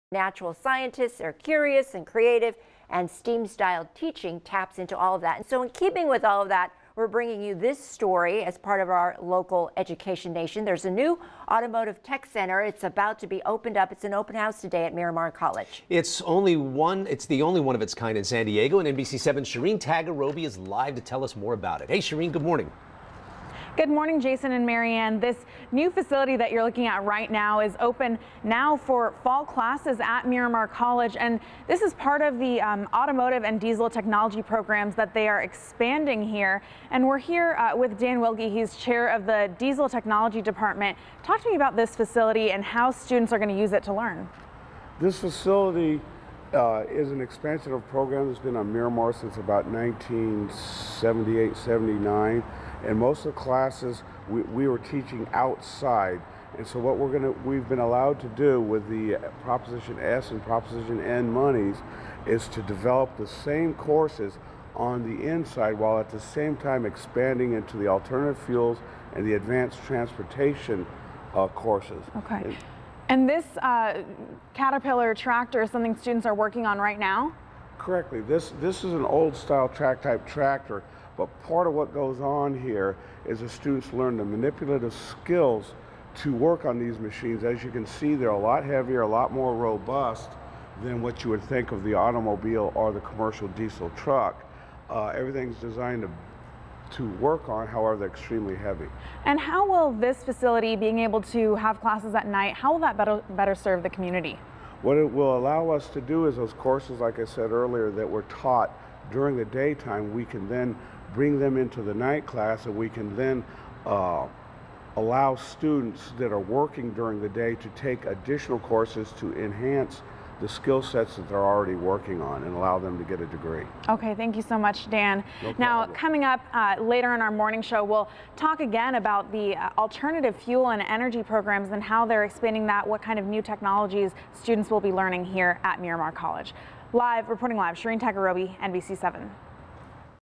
Miramar College Diesel Tech Program on NBC San Diego 7 aired October 9, 2013.
Spotlight on San Diego Community College District's Miramar College Diesel Tech Program on NBC San Diego 7, aired October 9, 2013.